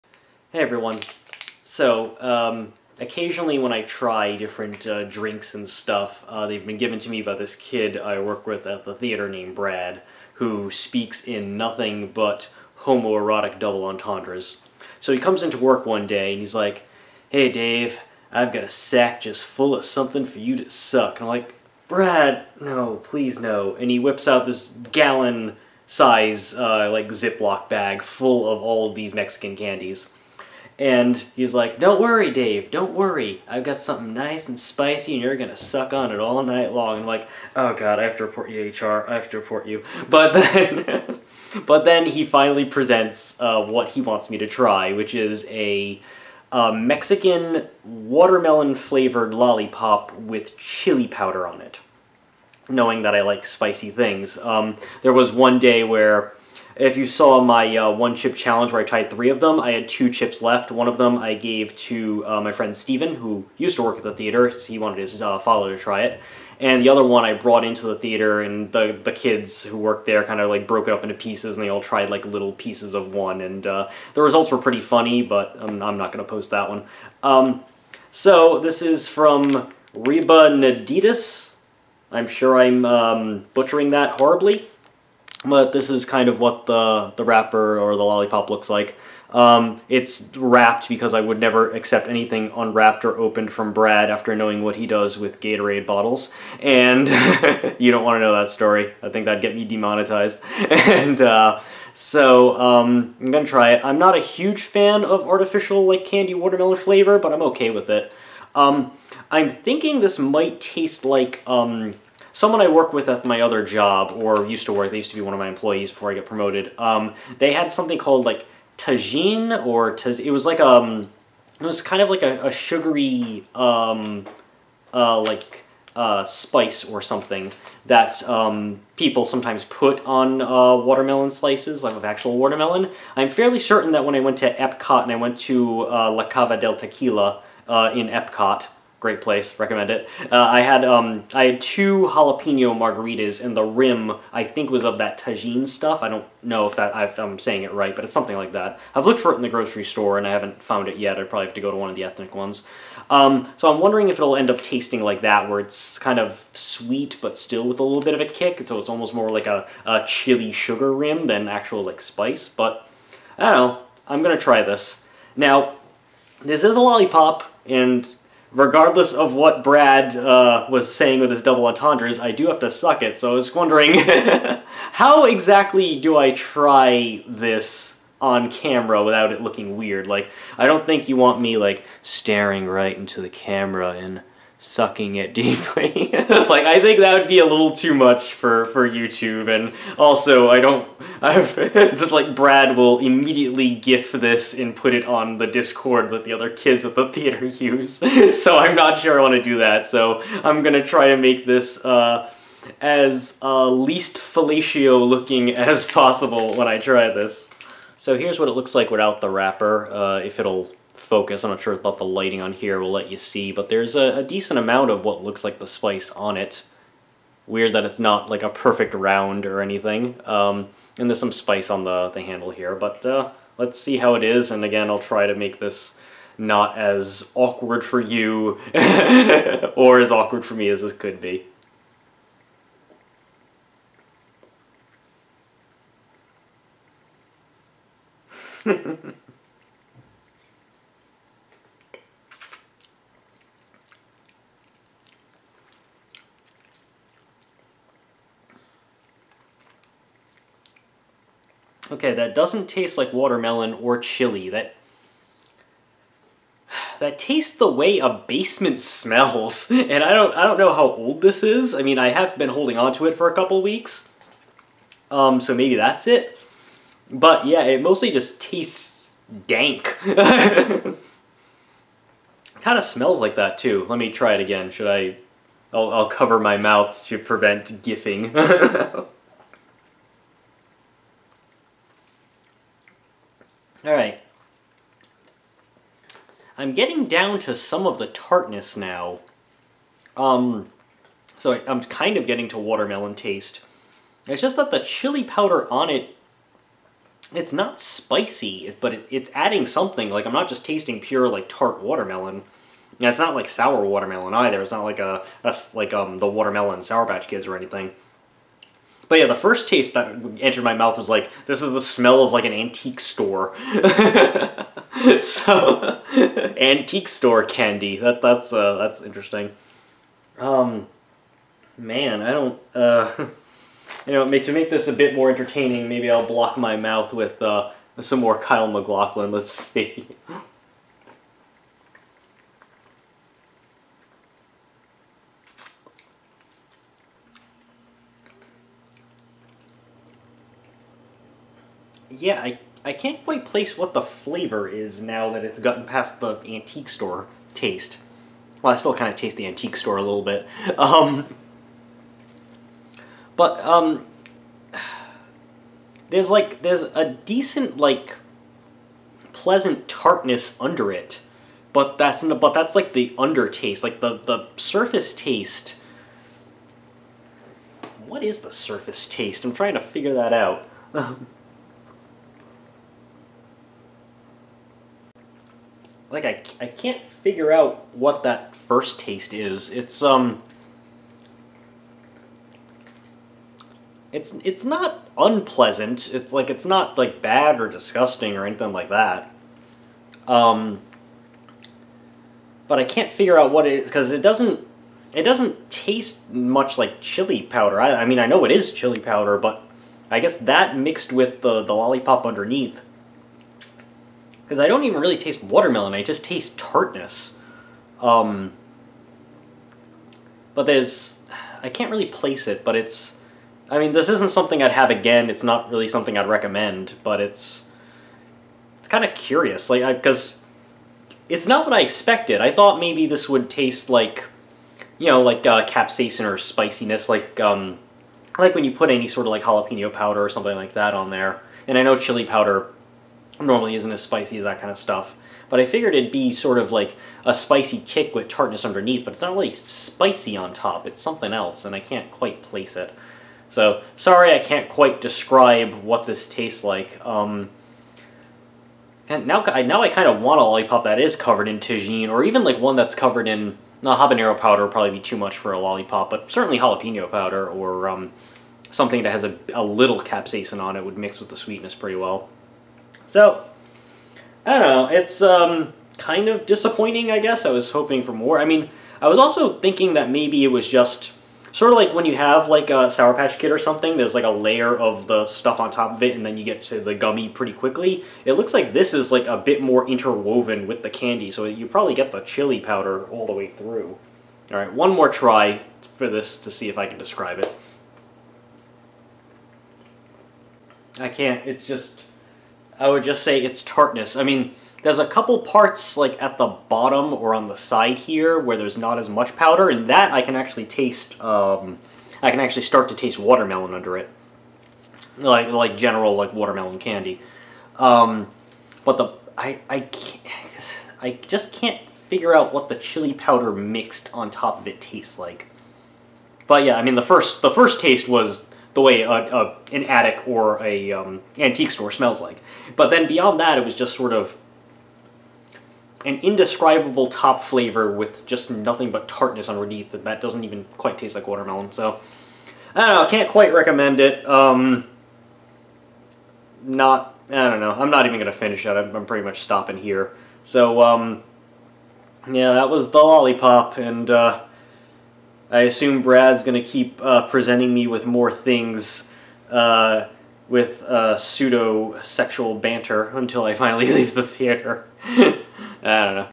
Leninhawk's Trying: Vero Rebanaditas Watermelon Lollipop w/ Chile Powder
Brad from the theater has given me something else to try, presenting it to me with homoerotic double entendres galore! While attempting to film this in a way that would keep Brad from photoshopping dicks into my mouth for GIFs, I try this Mexican candy lollipop with chile powder on it. I was expecting it to taste like Tajin but...